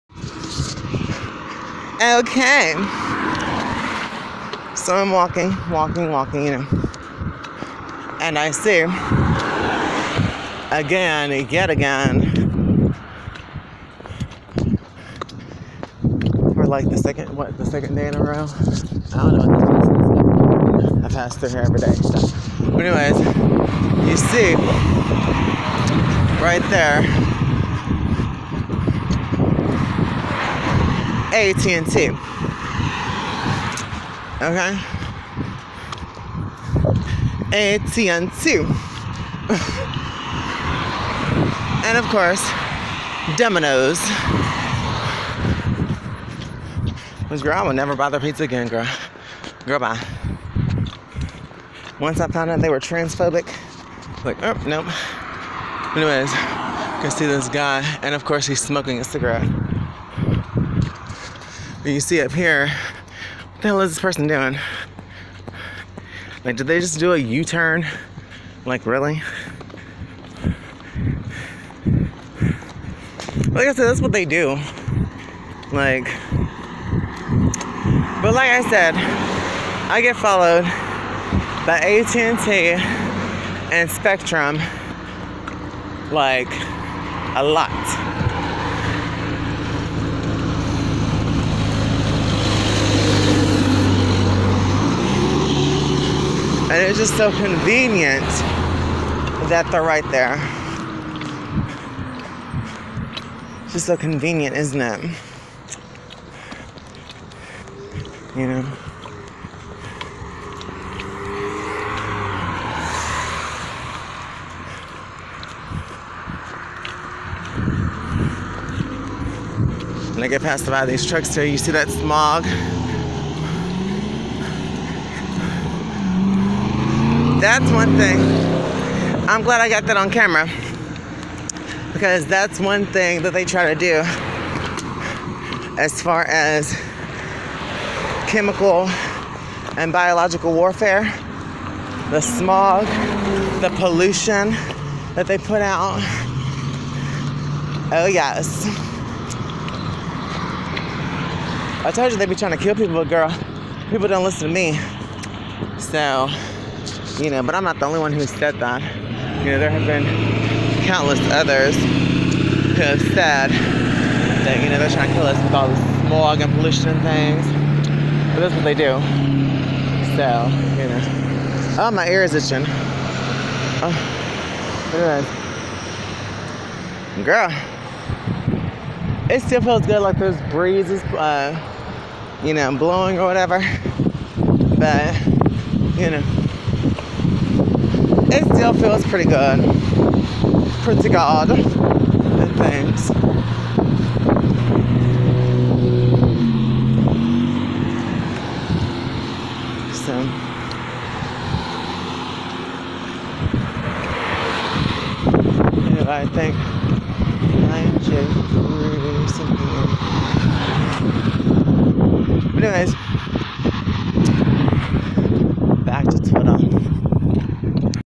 Journey from work #5 July 13th, 2018
Ive said it time and time again. I get followed by AT&T and SPECTRUM (TIME WARNER CABLE 👁) constantly. A van was there yesterday. And they were there again today.  Cannot be purely coincidence.